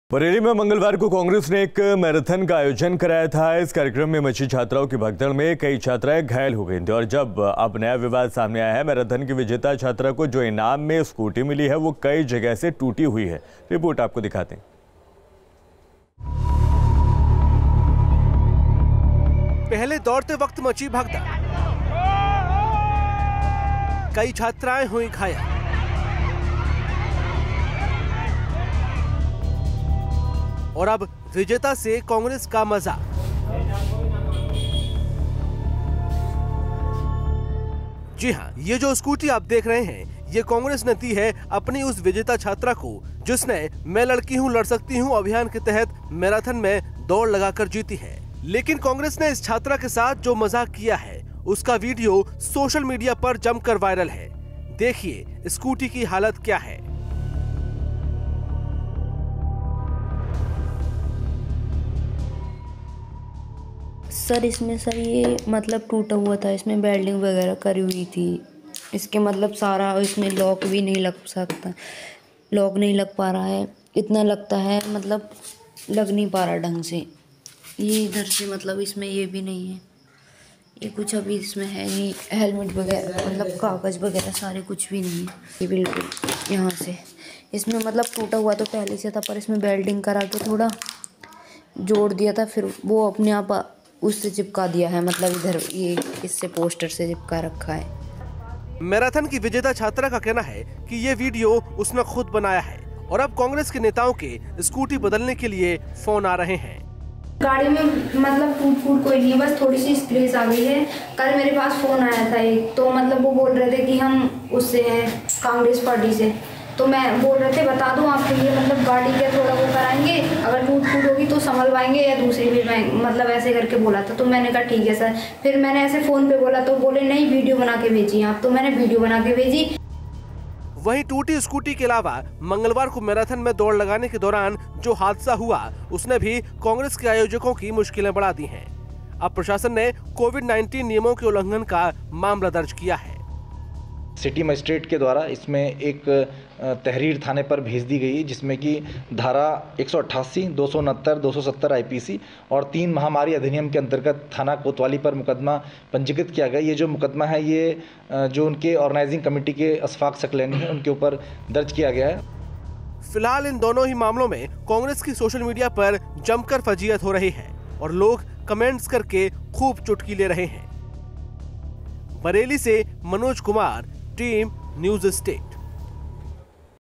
बरेली- कांग्रेस मैराथन में भगदड़, कई छात्राएं घायल
Bareilly Marathon Rally: बरेली में आयोजित कांग्रेस की मैराथन रैली में अचानक भगदड़ मच गई. बताया गया कि इस दौरान अव्यवस्था के चलते कई छात्राओं को चोट भी आई है. इस भगदड़ के बाद सड़क पर बच्चियों के जूते-चप्पल बिखरे पड़े नजर आए. इस घटना के बाद कांग्रेस नेताओं ने साजिश की ओर इशारा किया है. दरअसल कांग्रेस के लड़की हूं, लड़ सकती हूं अभियान के तहत ये मैराथन बरेली में आयोजित की गई थी. जिसमें तमाम छात्राओं को बुलाया गया था. भीड़ बढ़ने के चलते अव्यवस्था फैलती गई और अचानक भगदड़ मच गई.

News State Apps डाउनलोड करें:
Download Android and iOS Apps for more updates: